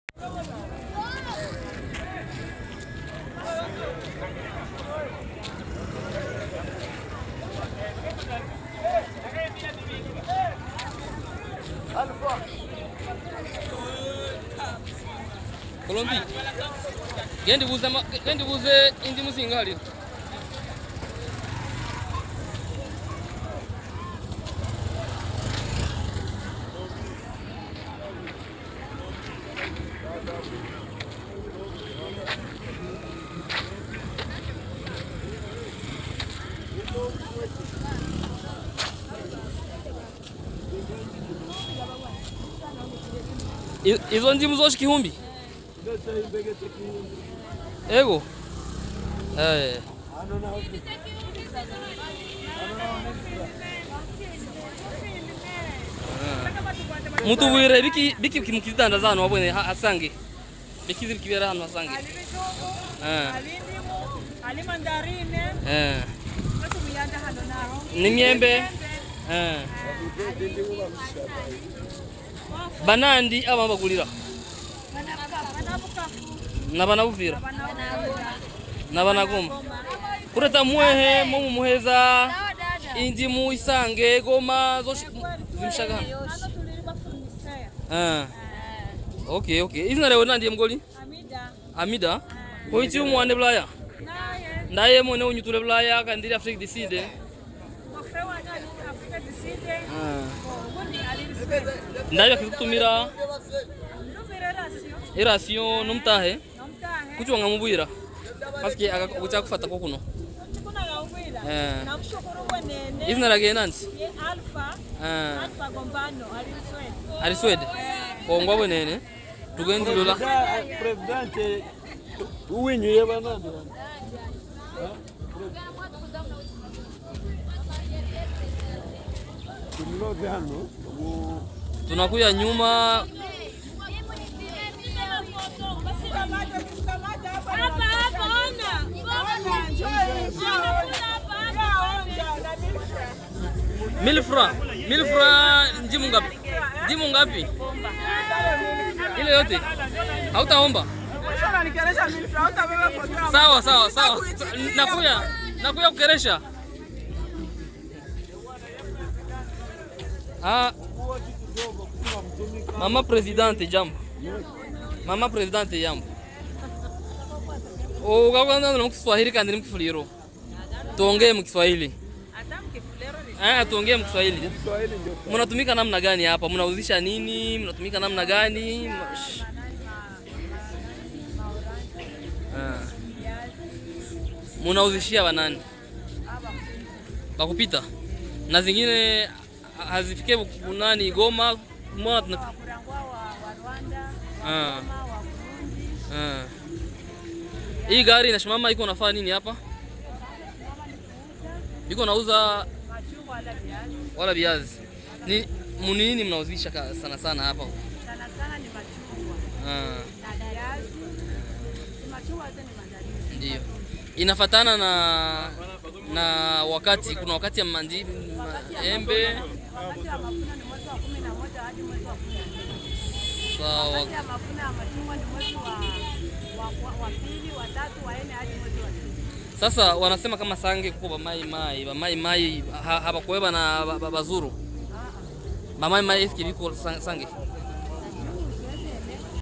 Copie de Sange, Uvira-RDC kasoko yako balabala
Une petite balade ku Kasoko yaku balabala pa Sange. Marché yenye inasaidia batu yakupita kutoka Mulongwe kwenda Bukavu et le contraire.
Video ya contributeur wa UviraOnline Ainsi Mulinzi.